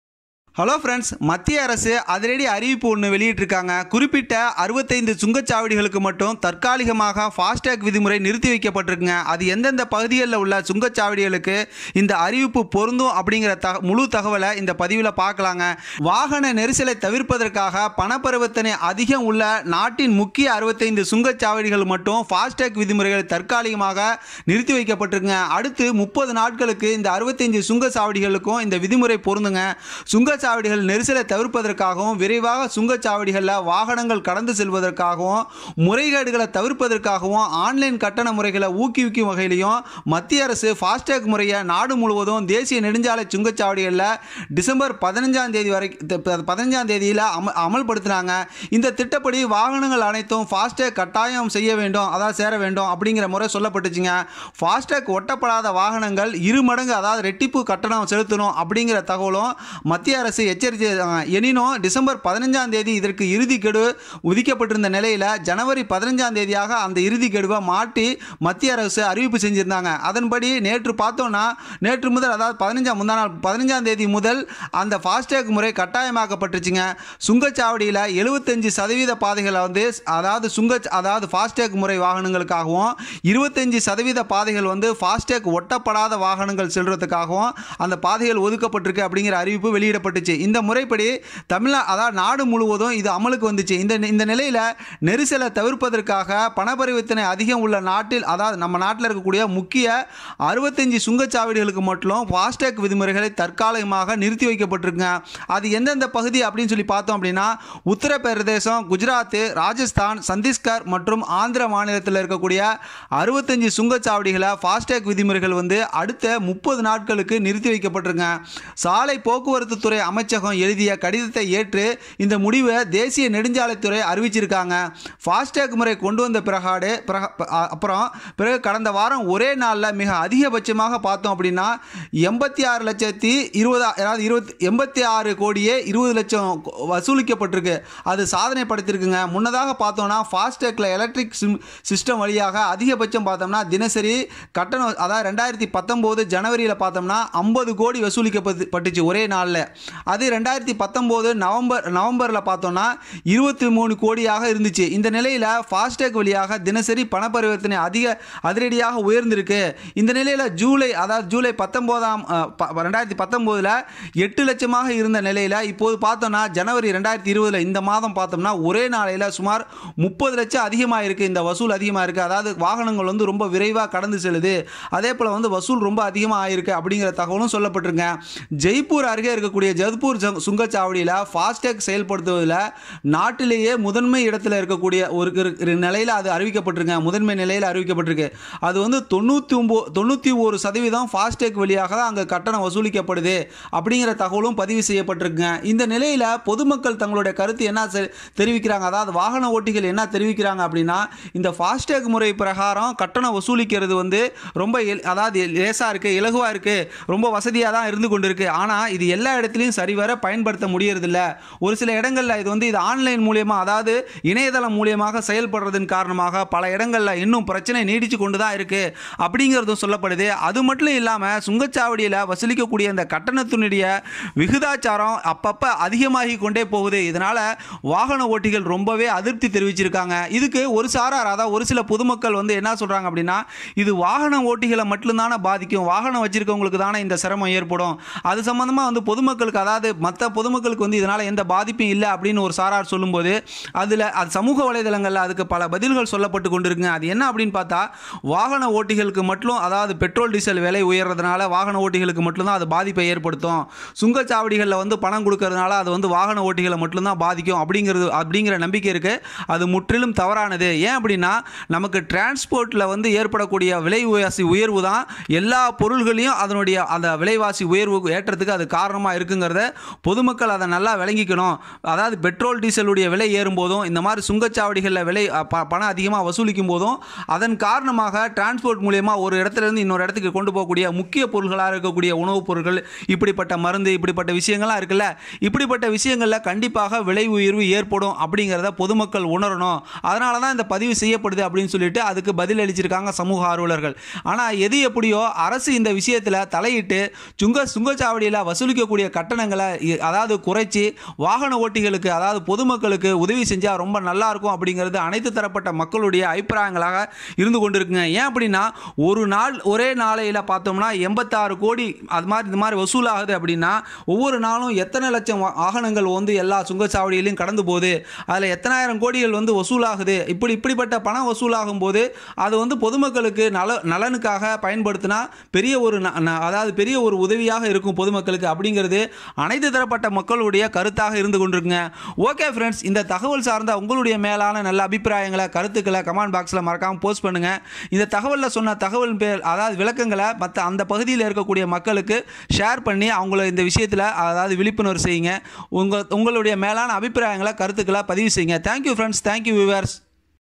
மத்திய அரசு அறிவிப்பு குறிப்பிட்ட 65 சுங்கச்சாவடிகளுக்கு மட்டும் ஃபாஸ்டேக் விதிமுறை நிறுத்திவைப்பு..
மத்திய அரசு அதிரடி அறிவிப்பு குறிப்பிட்ட 65 சுங்கச்சாவடிகளுக்கு மட்டும் ஃபாஸ்டேக் விதிமுறை நிறுத்திவைப்பு முழு விபரம் உள்ளே Hpastek.

இந்த திட்டப்படி வாகனங்கள் அனைத்தும் பாஸ்டேக்கில் கட்டாயம் சேர வேண்டும். பாஸ்டேக் ஒட்டாத வாகனங்கள் இரு மடங்கு கட்டணத்தை செலுத்த வேண்டும் என்று அரசு எச்சரித்தது. எனினும் டிசம்பர் 15ம் தேதி இதற்கு இறுதி கெடுவை ஜனவரி 15 ஆக மத்திய அரசு மாற்றியது. அதன்படி நேற்று முதல் பாஸ்டேக் முறை கட்டாயமாக்கப்பட்டுள்ளது.

சுங்கச்சாவடிகளில் 75 சதவீத பாதைகள் பாஸ்டேக் ஒட்டிய வாகனங்களுக்கும், 25 சதவீத பாதைகள் பாஸ்டேக் ஒட்டாத வாகனங்களுக்கும் என்றும் அறிவிக்கப்பட்டுள்ளது. இந்த முறை திட்டப்படி நாடு முழுவதும் அமலுக்கு வந்துள்ளது.

இந்நிலையில் நெரிசலை தவிர்ப்பதற்காக பணப்பரிவர்த்தனை அதிகம் உள்ள நாட்டின் 65 சுங்கச்சாவடிகளில் மட்டும் பாஸ்டேக் விதிமுறைகள் தற்காலிகமாக தளர்த்தப்பட்டுள்ளது.

உத்தரப்பிரதேசம், குஜராத், ராஜஸ்தான் சத்தீஸ்கர் மற்றும் ஆந்திர மாநிலங்களில் உள்ள 65 சுங்கச்சாவடிகளில் பாஸ்டேக் விதிகள் அடுத்த 30 நாளுக்கு தளர்த்தப்பட்டுள்ளது. சாலைபோக்குவரத்து துறை அமைச்சகம் எழுதிய கடிதத்தை ஏற்று இந்த முடிவினை தேசிய நெடுஞ்சாலைதுறை எடுத்துள்ளது.

பாஸ்டேக் முறை கொண்டுவந்த பிறகு கடந்த வாரம் ஒரே நாளில் மிக அதிகபட்சமாக ரூ.86.2 கோடி வசூல் ஆகி சாதனை படைத்தது. முன்னதாக ஃபாஸ்டேக் எலக்ட்ரானிக் சிஸ்டம் வழியாக அதிகபட்ச தினசரி கட்டண வசூல் 2019 ஜனவரியில் ரூ .50 கோடியாக (ஒற்றை நாள் வசூல்) 2019 நவம்பரில் 23 கோடி ரூபாயாகவும் இருந்தது.